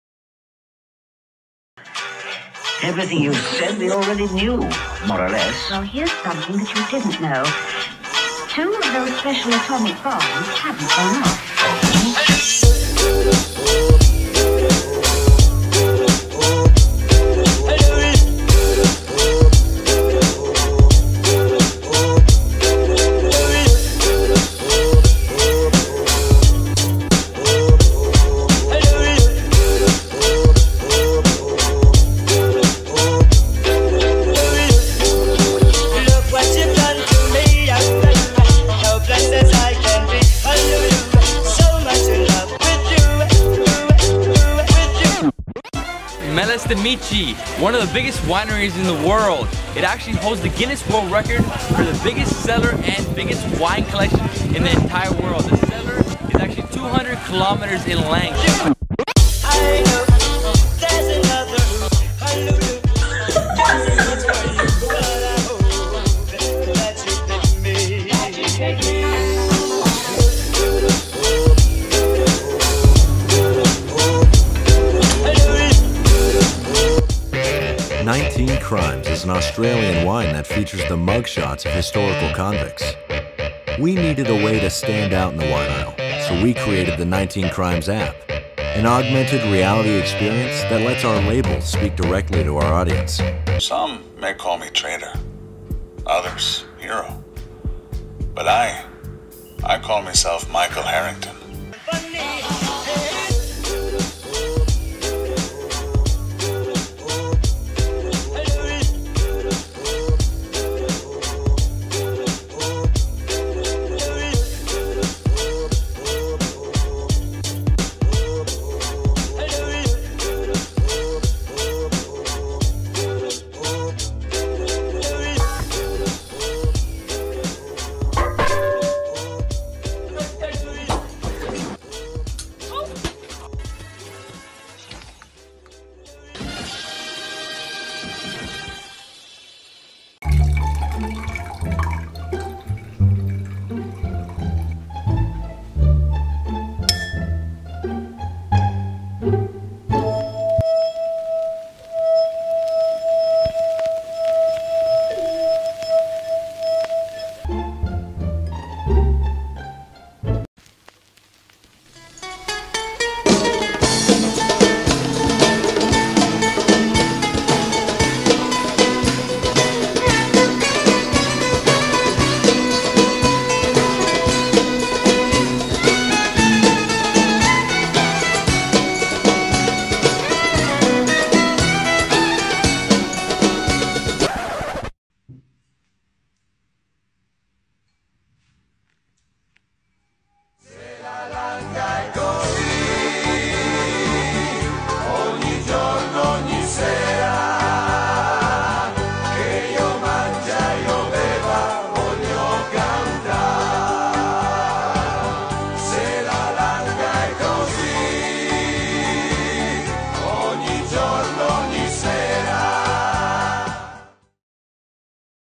🍷 Wine People are Awesome 🔥 (Best Funny, Viral Wine moments)
🍷 Wine People are Awesome 🔥

We compiled the funniest, most extraordinary and VIRAL 🍷 WINE MOMENTS (and some champagne moments) in one video for all you winelovers out there!

A big thank you to all the content creators for their amazing content and innovators like 19 Crimes & Thompson Woodworks for thinking outside the box!

👉 Tag a winelover friend in the comments and let us know which was your favorite wine moment...